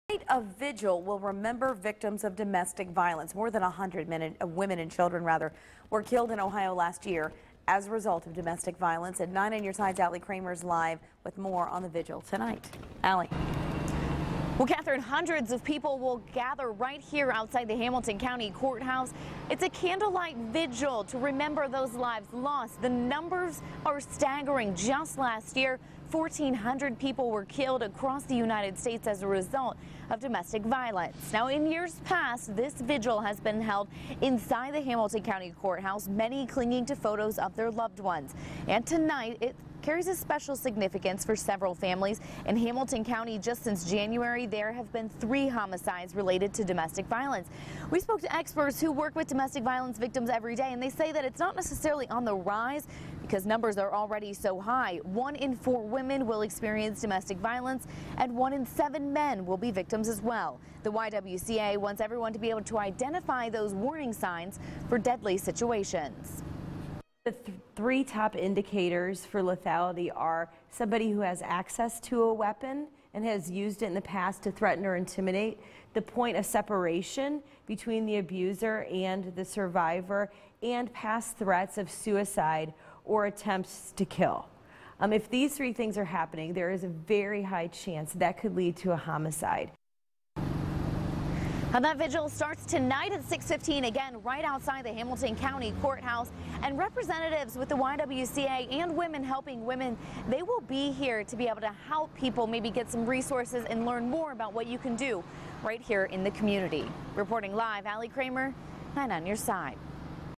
Candlelight vigil for domestic violence victims
Vigil occurs at 6:15 p.m. Tuesday at the Hamilton County courthouse.